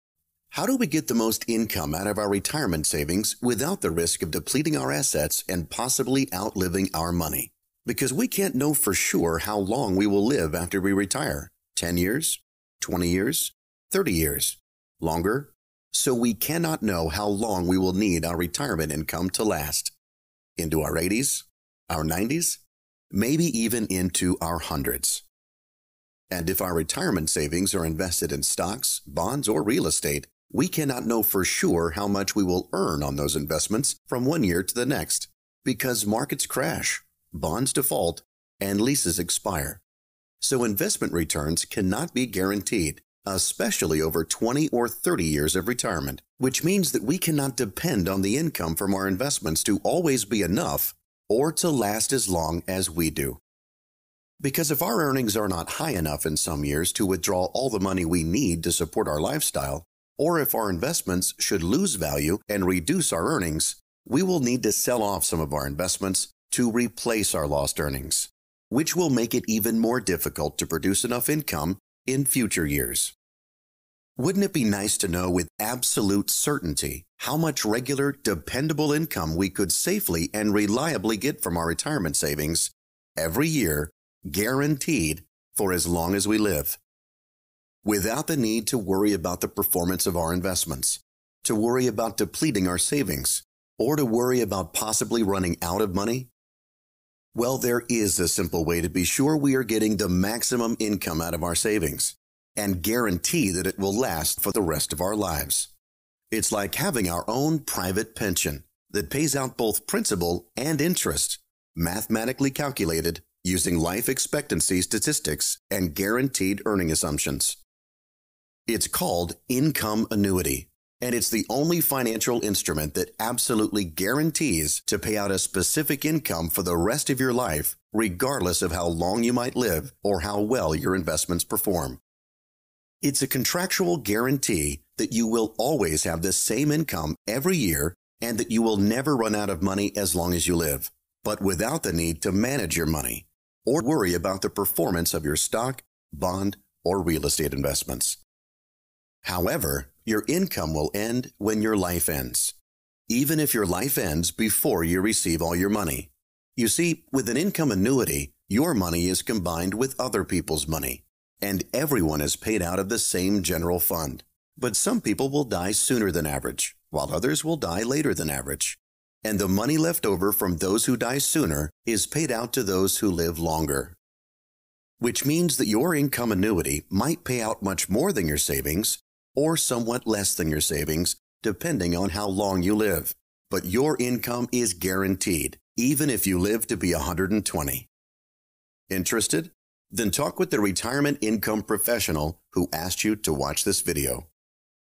Guaranteed Lifetime Income Without Investment Risk
If you are retired or approaching retirement, and you want a guaranteed monthly income you can never outlive, watch this video to learn how you can convert any amount of retirement savings into a guaranteed pay check for the rest of your life.

Every person’s situation varies, so figuring out what you need boils down to you deciding what your family needs in the event you pass away.